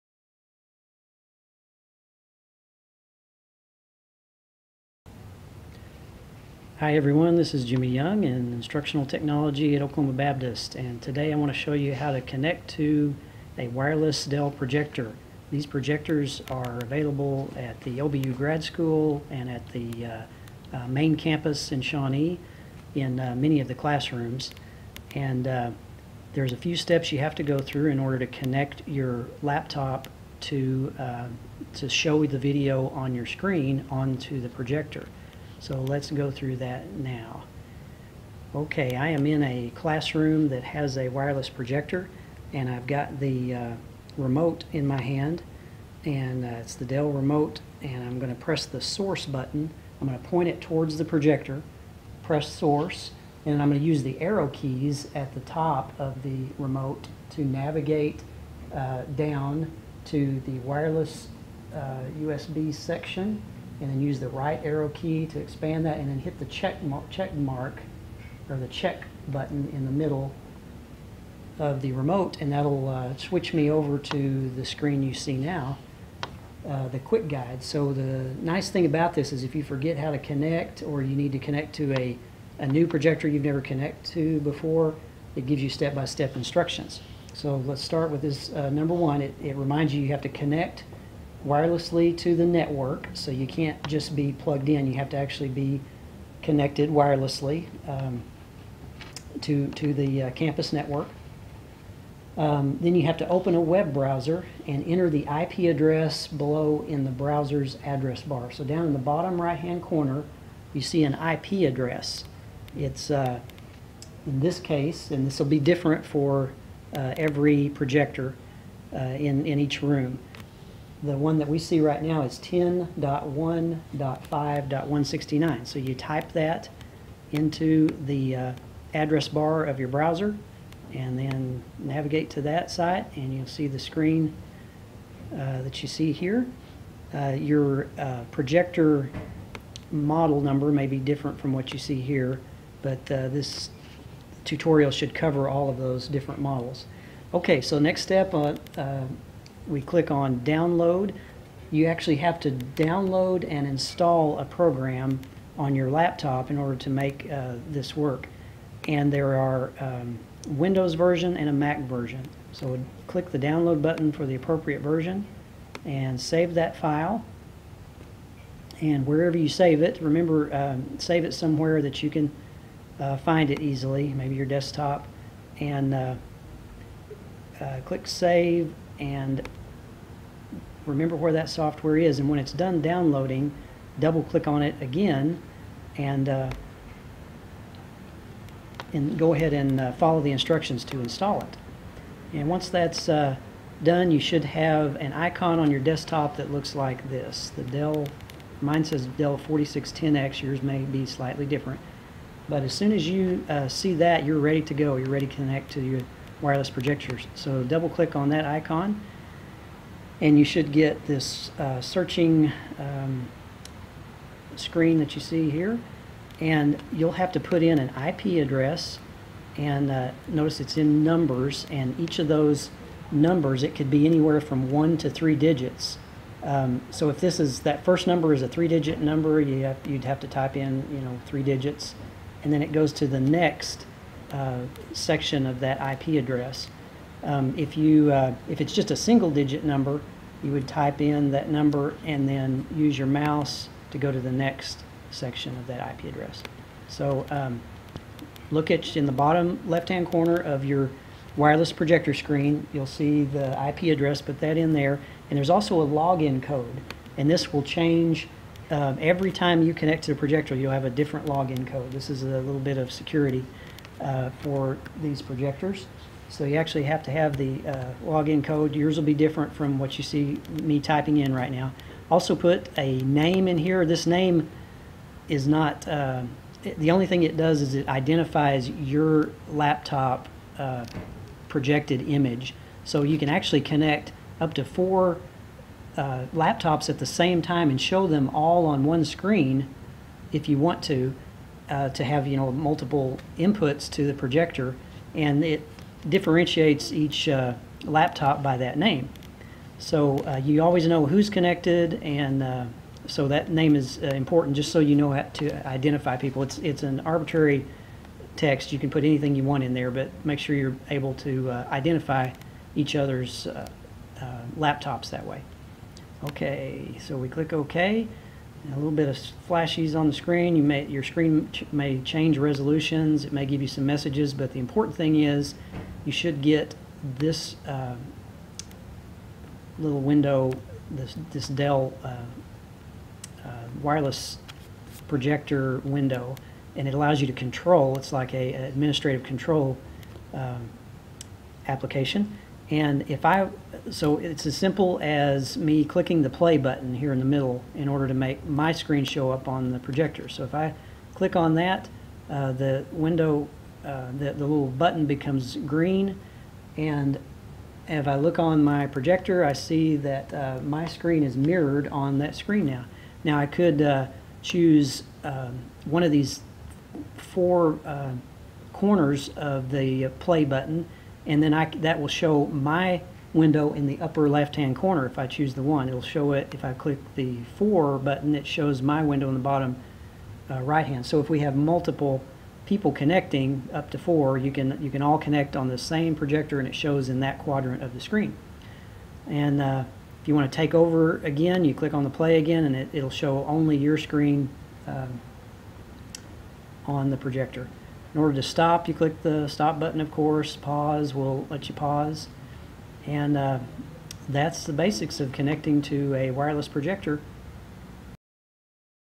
Dell Wireless Projector Tutorial - OBU
How to connect your laptop to a Dell wireless projector